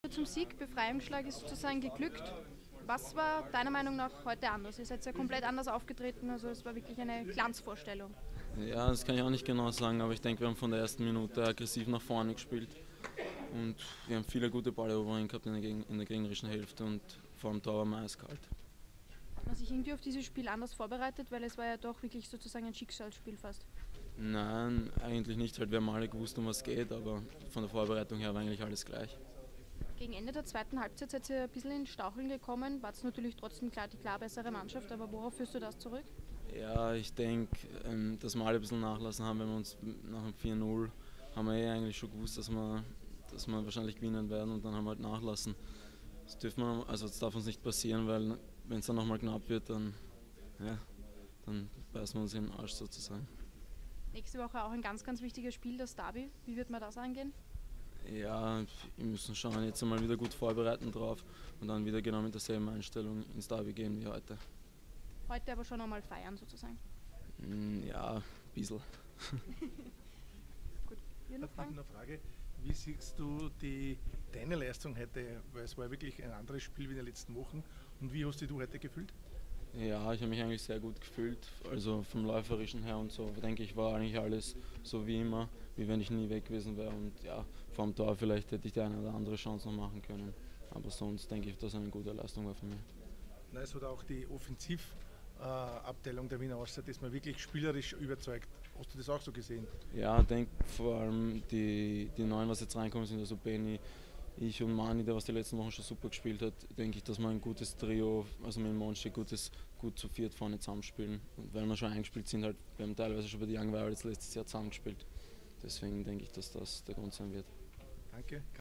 Dominik Fitz (Austria Wien) nach dem Spiel Austria Wien gegen TSV Hartberg
In der 16. Runde der Tipico Bundesliga trafen in der Generali Arena die Mannschaften von Austria Wien und TSV Hartberg aufeinander. Das Spiel endete 5:0.

Nach dem Spiel standen die Spieler 3-fach Torschütze Christoph Monschein, Benedikt Pichler und Dominik Fitz den Journalistinnen und Journalisten Rede und Antwort.